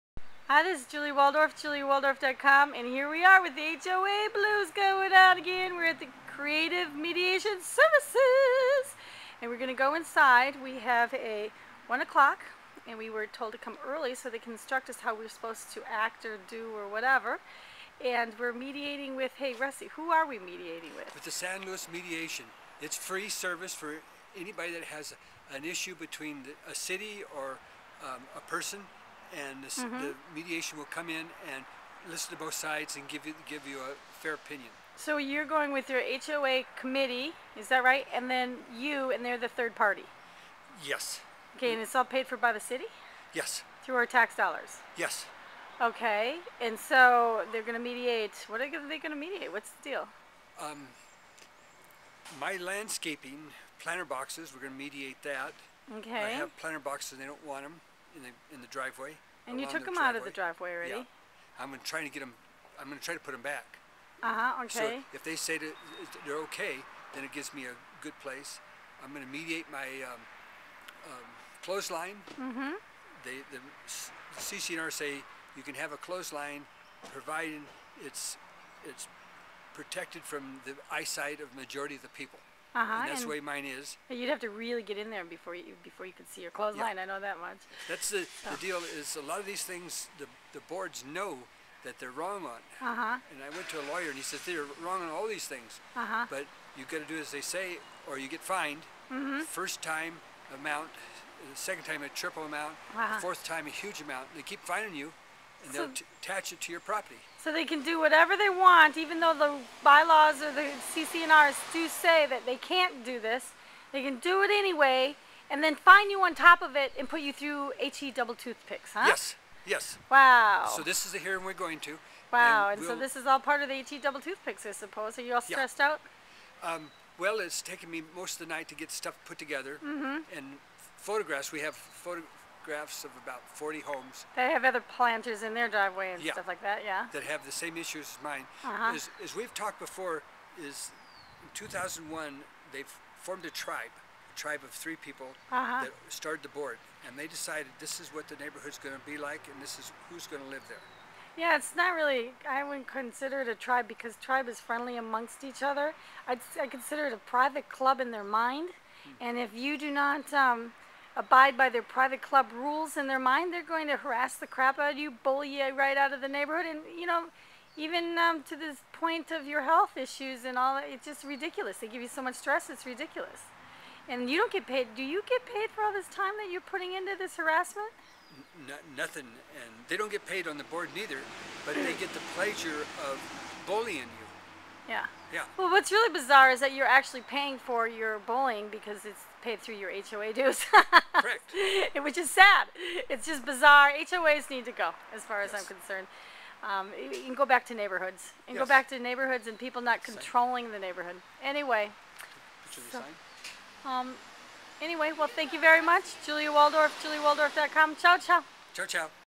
HOA BLUES part 4
interviews Russee Parvin about the home owners association issue he is having with his neighborhood home owners association. see this and more about real estate, fun in the sun, central coast california, homeless, single parents